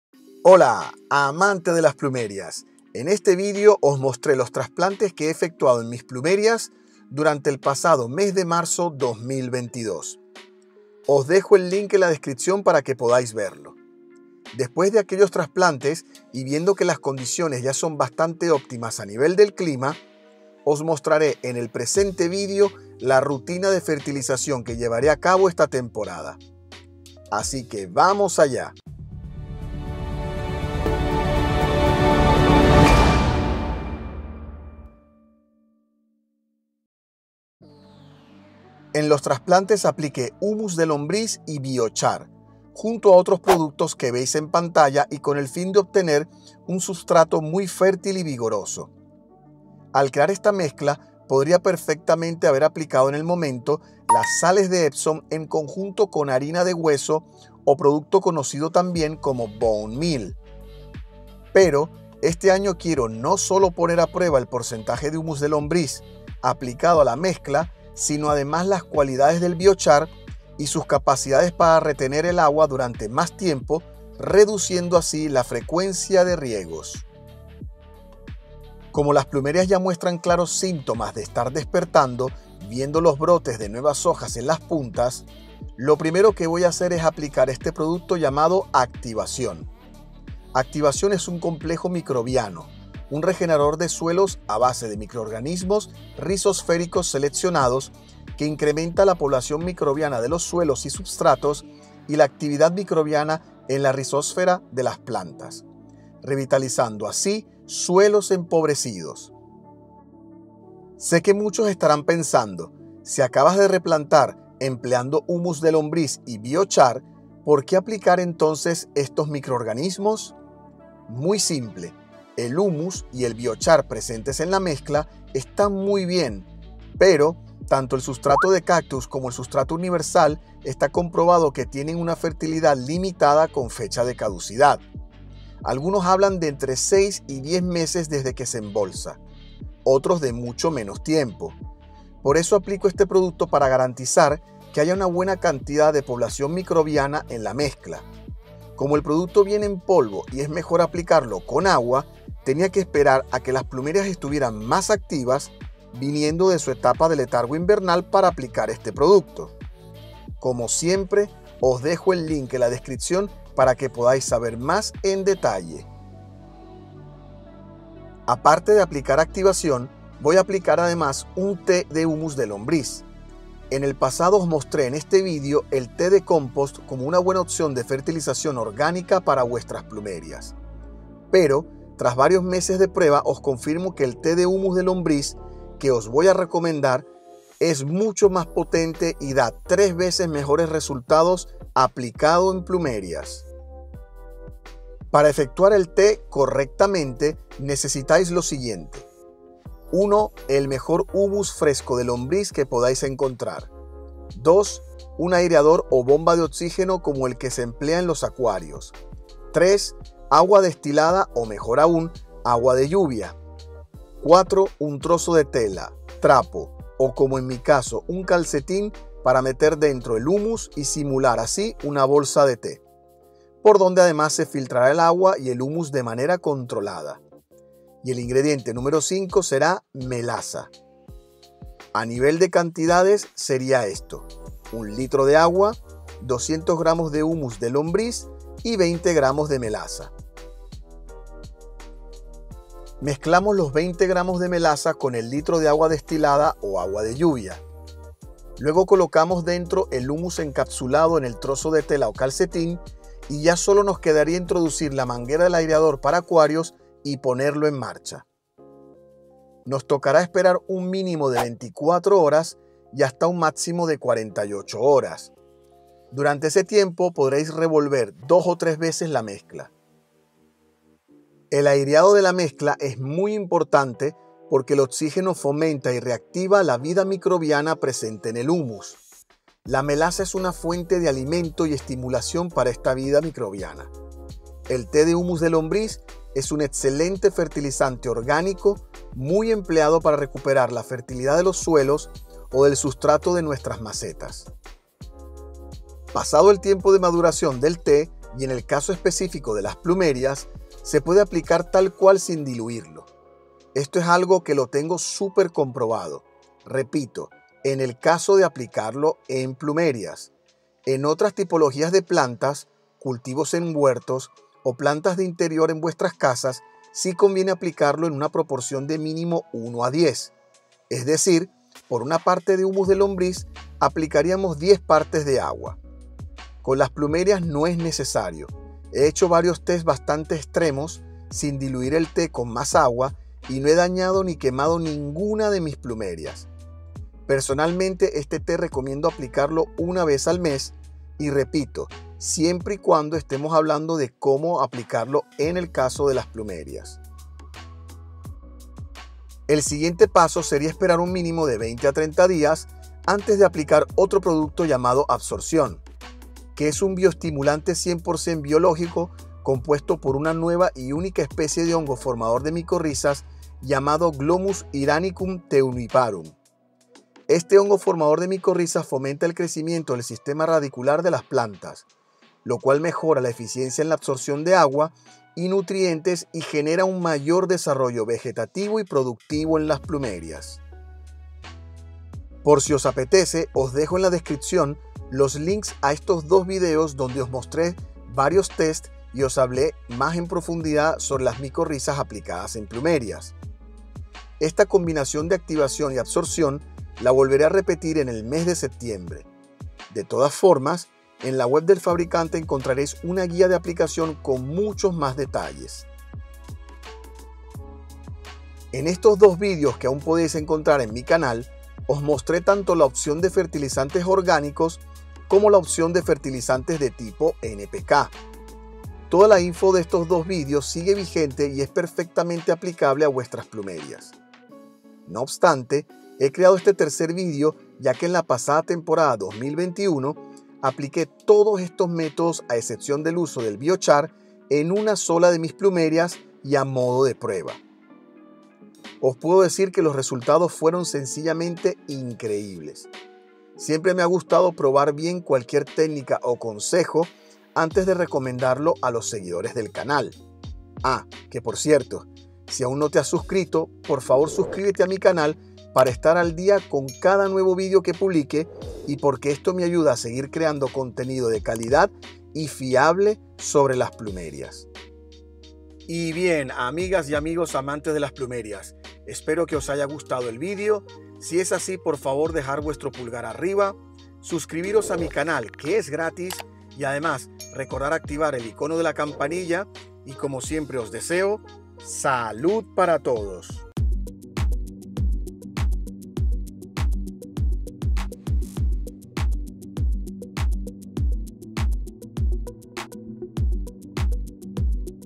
🤩 FERTILIZO mis PLUMERIAS - ¿Cómo lo hago? ¿Qué uso? - 😎⏰🚨
En este vídeo te muestro como fertilizaré mis plumerias en el inicio de la campaña en Mayo del 2022

IMPORTANTE: La foto de la flor de plumeria empleada en la miniatura de este vídeo es una foto creada por ©Copyright Kukiat Tanteeratarm - Nombre de la variedad: SPECKLED JEWEL.